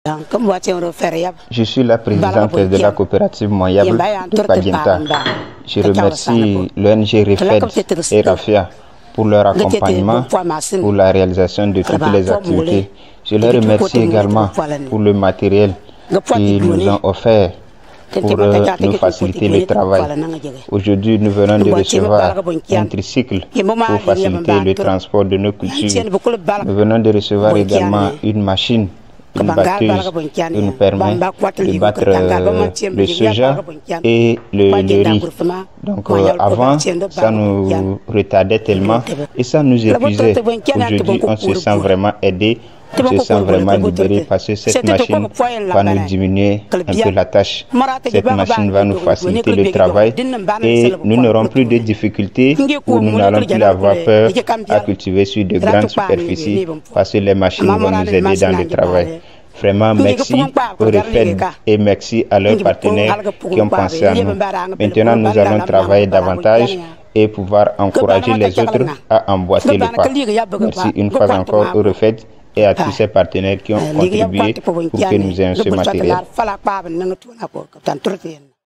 REFED SAVANE_ Programme d’appui à la prévention des conflits et de l’extrémisme violent
Une seconde bénéficiaire du Programme d’appui à la prévention des conflits et de l’extrémisme violent dans les zones frontalières du Bénin, du Burkina Faso et du Togo se prononce.